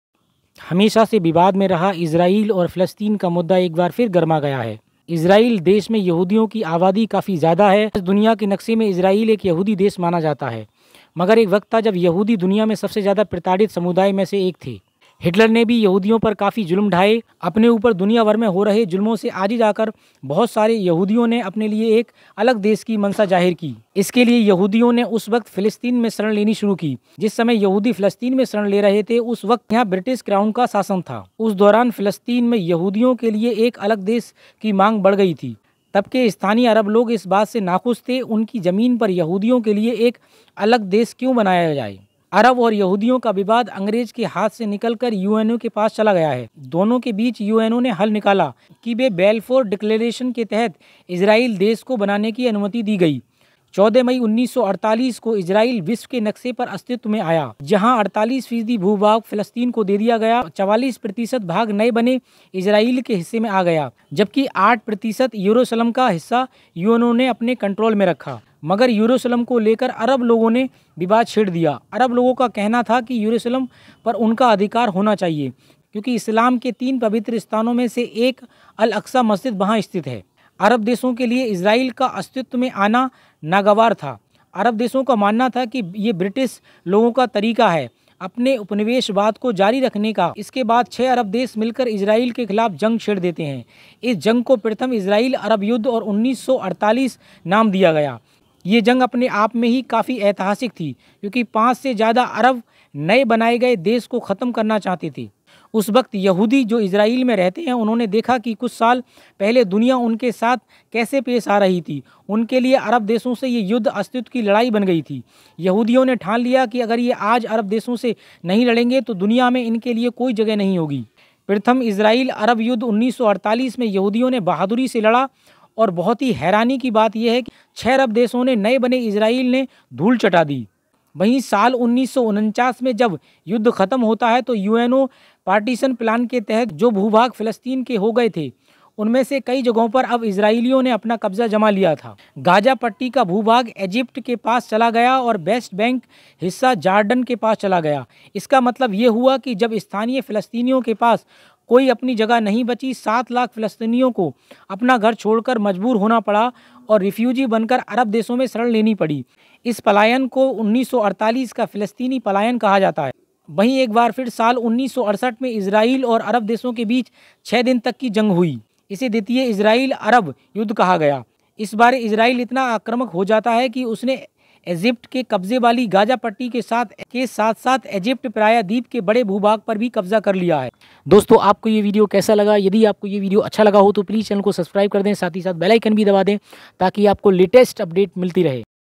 इजराइल-फिलिस्तीन के बीच विवाद का लंबा इतिहास, क्या है ताजा संघर्ष की वजह Israel-Palestine dispute
इजराइल-फिलिस्तीन के बीच विवाद का लंबा इतिहास, जानिए- क्या है ताजा संघर्ष की वजह
Israel-Palestine dispute
इजराइल-फिलिस्तीन विवाद